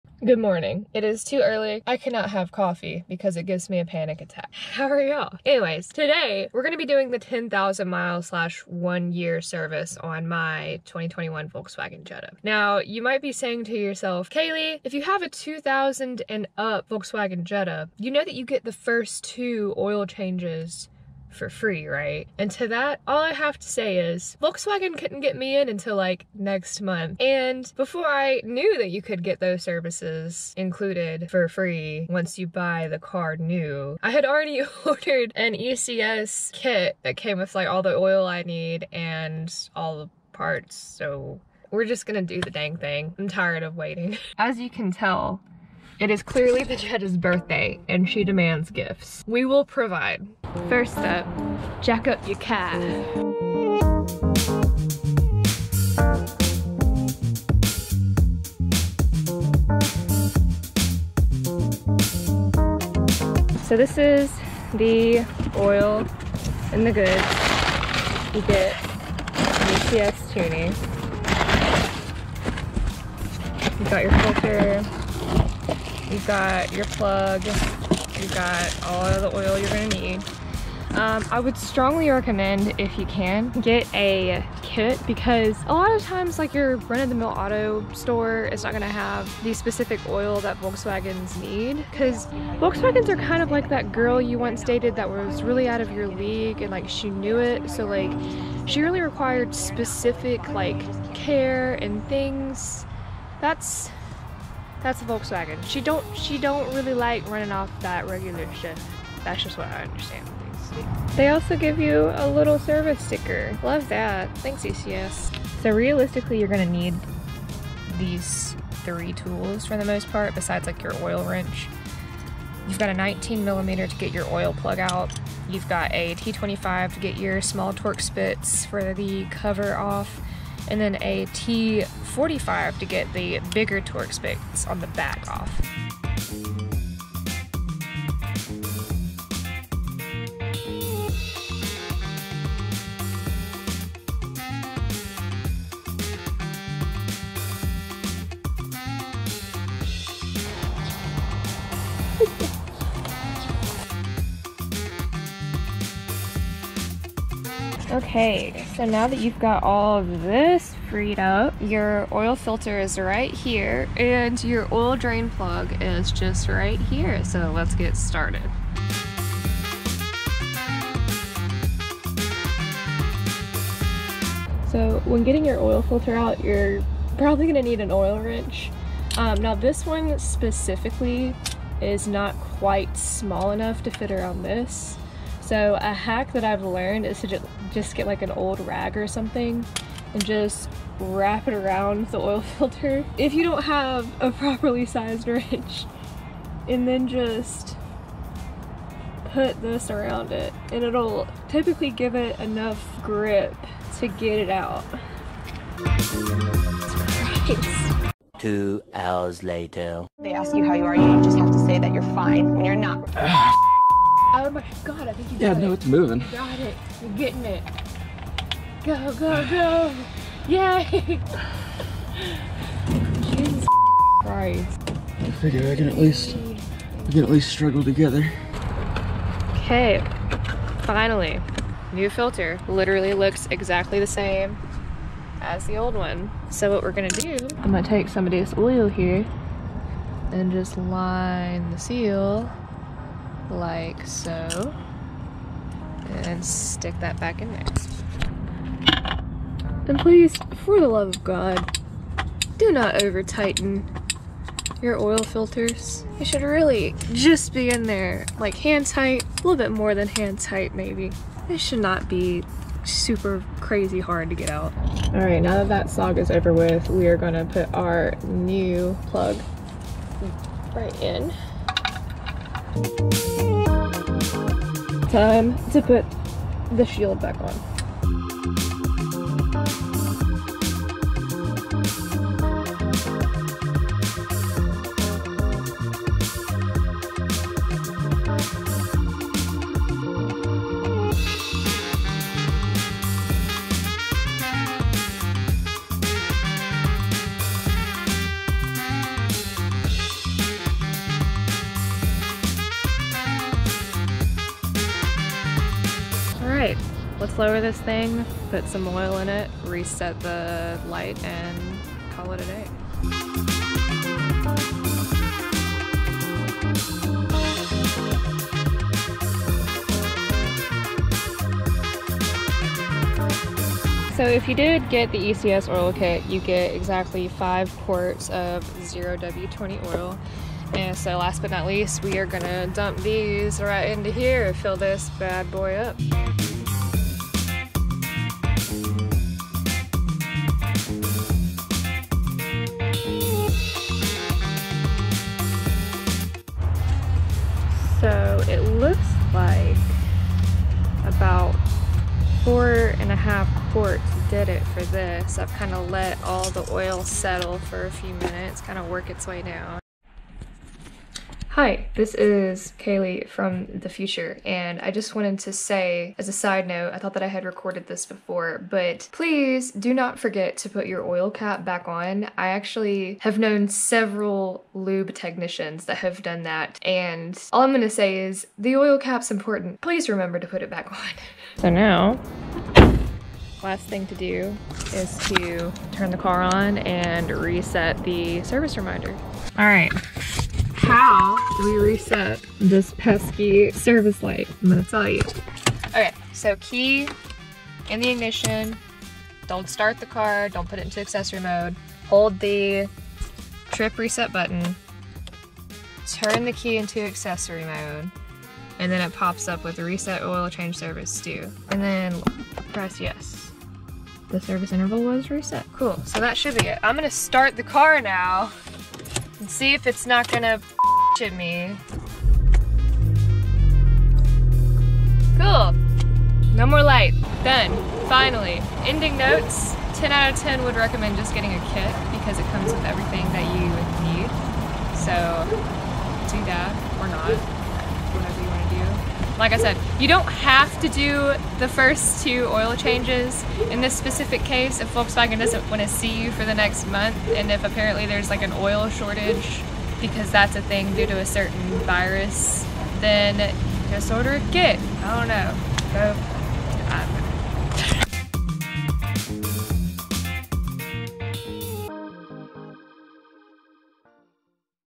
MK7 Jetta Oil Change
I change the oil on my 2021 Jetta for the first time.
NOTE: If you bought a 2020 and up Volkswagen new, the first two services are included for free. (I just did mine myself because I couldn't get scheduled with the dealer for another month.)
Link to the oil kit I purchased: